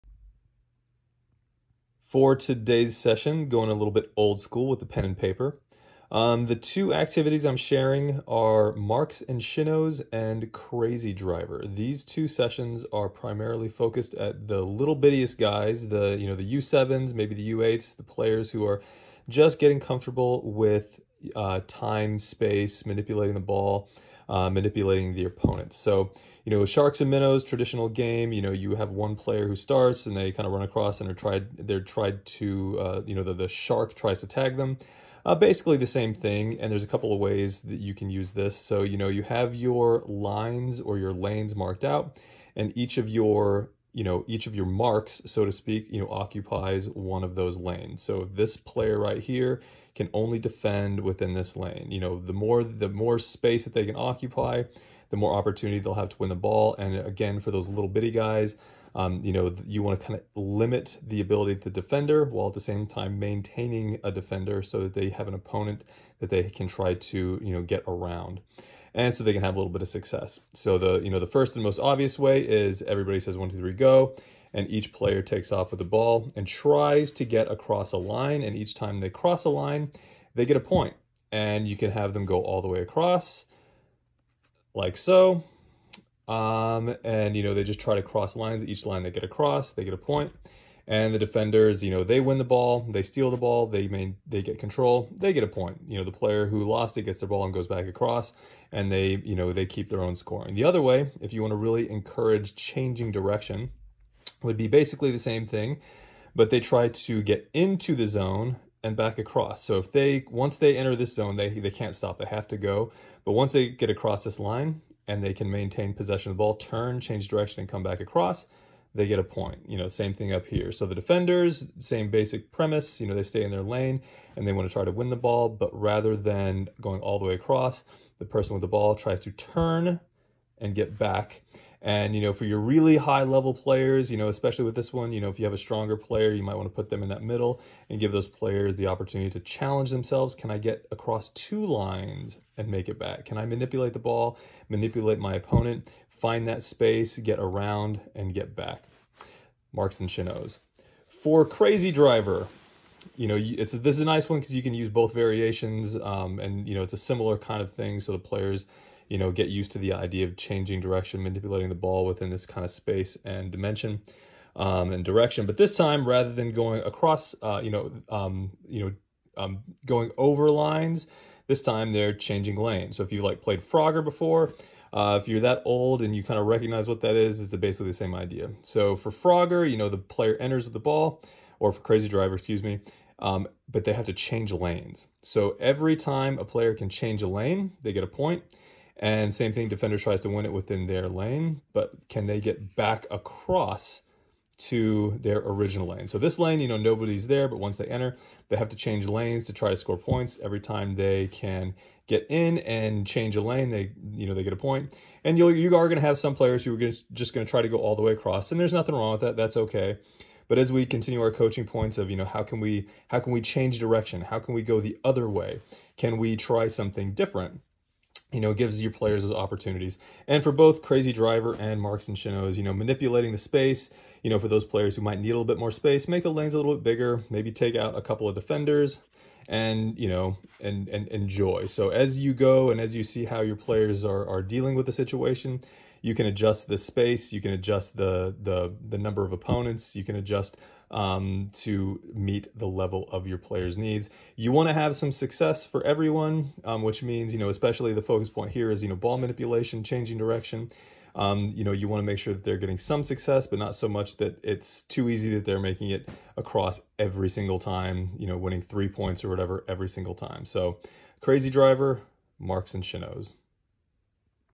Crazy Driver! & Marks and Shinnows- U7 games
Two variations on some classic games for beginning players that include:
-Direction
-Dimension
-Opponents